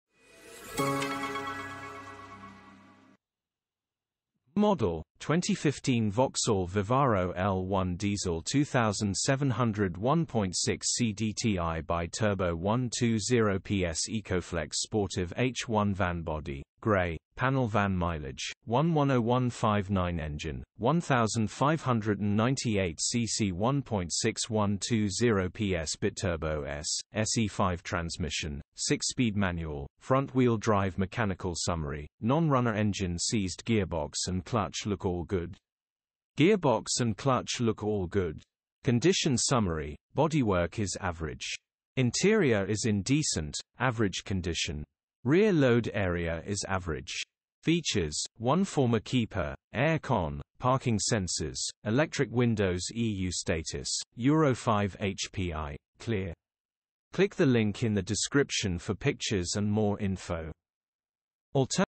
ET15XGK - 2015 VAUXHALL VIVARO L1 DIESEL 2700 1.6CDTI BiTurbo 120PS ecoFLEX Sportive H1 Van
Model: 2015 VAUXHALL VIVARO L1 DIESEL 2700 1.6 CDTI BI TURBO 120PS ECOFLEX SPORTIVE H1 VAN
Body: Grey - Panel Van
Mileage: 110159
Engine: 1598cc 1.6 120PS BITURBO S/S E5
Transmission: 6 Speed Manual - FWD
Mechanical Summary: 🔴 Non runner- Engine Seized- Gearbox and clutch look all good. Gearbox and clutch look all good.
Condition Summary: Bodywork is average.  Interior is in decent, average condition. Rear load area is average.
Features: 1 Former Keeper, A/C, Parking Sensors, Electric Windows
EU Status: Euro 5
HPI: Clear